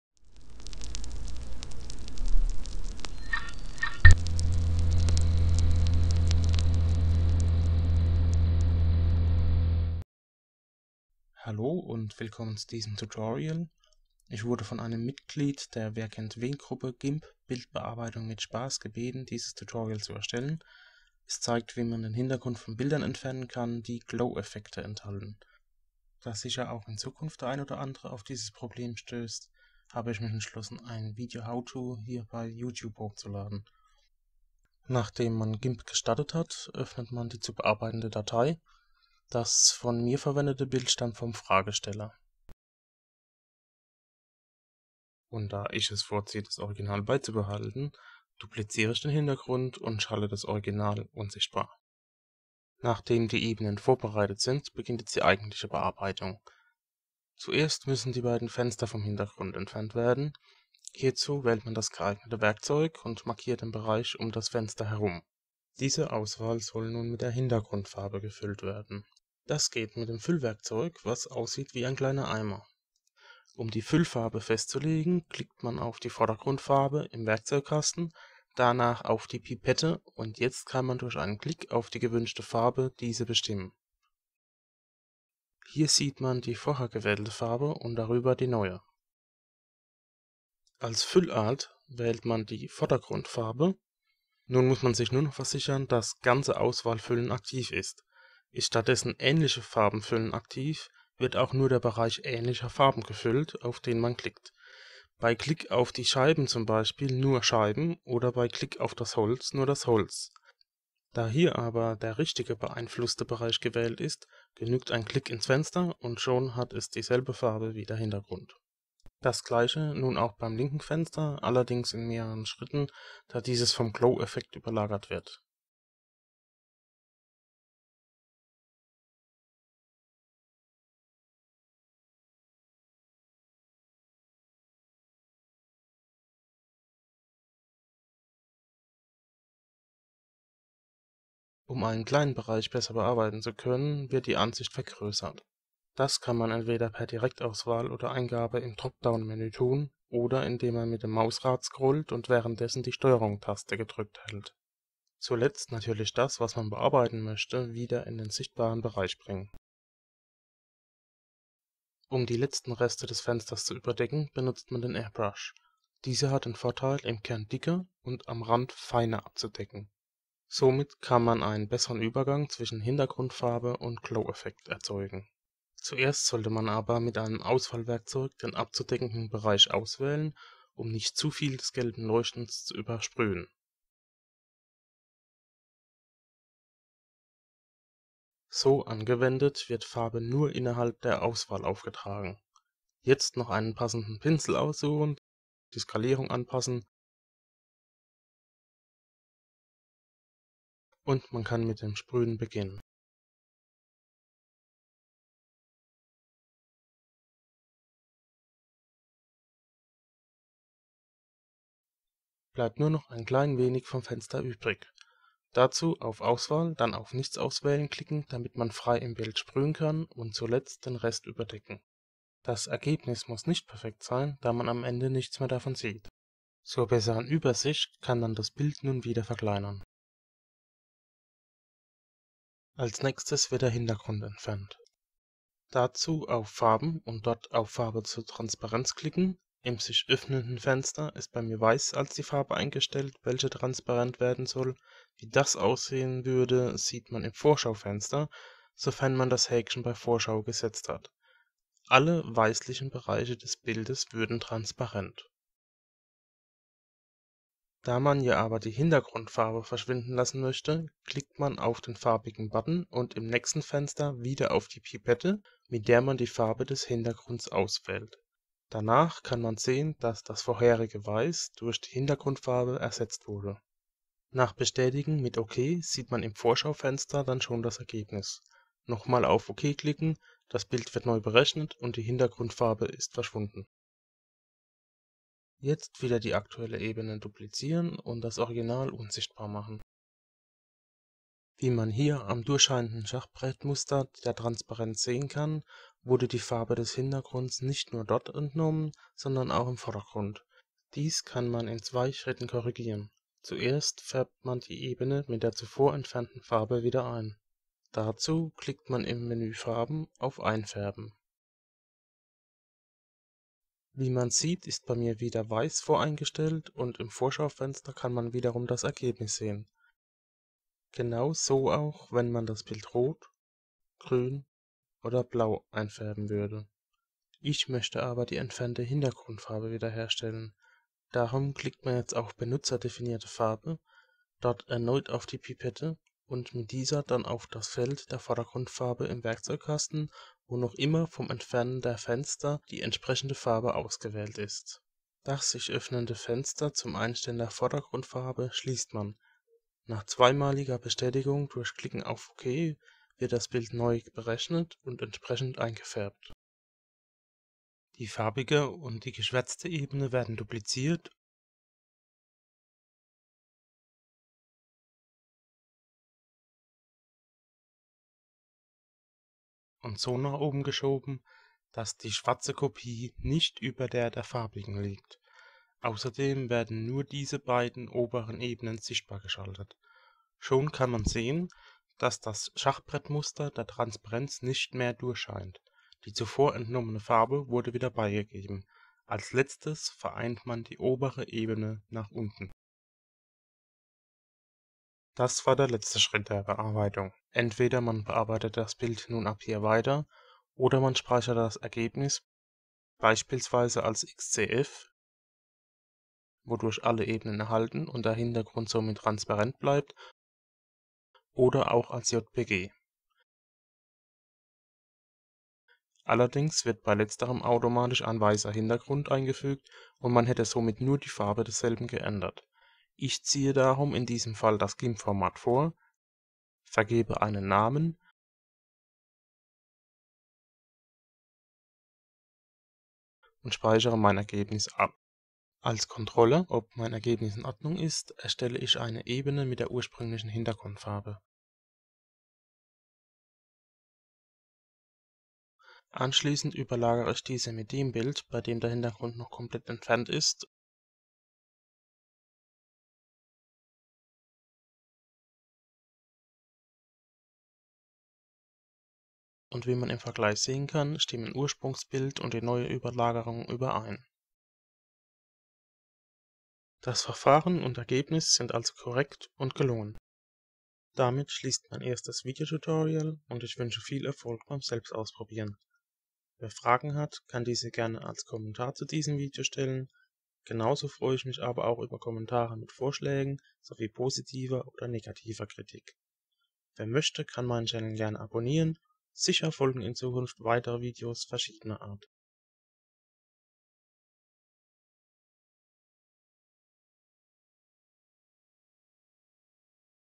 Gimp: Remove Background from Picture w/ Glow-Effects (german, description for english) [Tutorial 1]
This is a GIMP Tutorial.    SEE THIS DESCRIPTION FOR THE ENGLISH VERSION ABOUT HOW TO REMOVE THE BACKGROUND, STEP BY STEP, FROM A PICTURE THAT HAS GLOW EFFECTS IN IT.

The picture was supplied by the enquirer who originally asked me to make this tutorial:

Step 1 (0:37):
Start GIMP and open the file.

Step 2 (0:45):
Always keep the original. Duplicate the picture and make the original invisible.

Step 3 (0:52):
First remove the two windows from the background. Choose a selection tool, draw a frame around the right window. Then fill the selection with the "Bucket
Fill Tool". To choose the fill colour, click on the foreground colour in the toolbox, then on the eyedropper and after that, select the colour you want by just clicking on it.
As fill type choose the "FG colour fill". Make sure that "fill whole selection" is marked as the affected area. If instead "fill similar colours" is chosen, only similar parts within the selection you click on are filled. Since the right affected area is chosen, a click somewhere inside the selection is enough to give it the new colour.

Step 4 (2:00):
Now the left window in several steps because of the overlay of the window and the glow-effect.

Step 5 (2:22):
Zoom the view in. This can be done by either direct selection in  drop-down menu or by turning the mouse wheel while holding the ctrl-key. Bring the area to edit back in sight.

Step 6 (2:42):
To remove the last bit use the airbrush. It paints thicker in the middle but thinner to the edge. Thus a better transition between the background colour and glow effect can be made.
First mark the working area with a selection tool for not to overlay too much of the yellow glow. Now colour is only put on inside that selection. Choose a brush, adjust the scale and start spraying.

Step 7 (3:32.5):
Click on "Select", then "None" for freely spraying everywhere and spray the rest. The result must not be perfect - you wont see it later on.

Step 8 (3:46):
Zoom the picture out again.

Step 9 (3:52):
Click on "Colors" and "Color to Alpha". Here white is chosen as the colour which will be transparent. You can see how that would look like in the preview window if you have checked "Preview". All the white would be gone.
Click the coloured button, then the eye dropper and choose the backgrounds colour. After hitting "OK" you can see the new result in the preview window. Again
click "OK" and the background colour is gone.

Step 10 (4:45):
Duplicate the layer and make the original unvisible.

Step 11 (4:51):
By the sheer visible chequer of the transparency, you see that colour was not only removed from the background but from all of the picture.
To correct that click on "Colors" and "Colorify". Click on "Custom color", then the eye dropper, and with that on the foreground colour in the toolbox, where the right colour is still selected from painting over the windows. Close the now opening window. After hitting "OK" two times, the picture is beeing calculated and correspondingly colourized.

Step 12 (5:57):
Duplicate the coloured and the  monochrome layers, move them to the top so the monochrome layer beneath the coloured one. Make only those top layers visible.
The before abstracted colour is now added again. Finally merge top layer down.

Step 13 (6:37.3):
Either go on editing or you save the result e.g. as a XCF file (keeps all the layers), as PNG (keeps the background transparent) or as a JPG. JPG automatically adds white as background colour which you probably wont want. So I prefer the GNP-Format over JPG, type in a name and save the result.

Epilog (7:16):
For to check if the result is good, I create a new layer with the original background colour, overlay it the pictures where the background colour is removed from everywhere. As you can see in comparison, the original picture and this one do match. So my method and the result are good.

Conclusion (7:50):
This concludes my first tutorial and I wish you good luck trying it out on your own.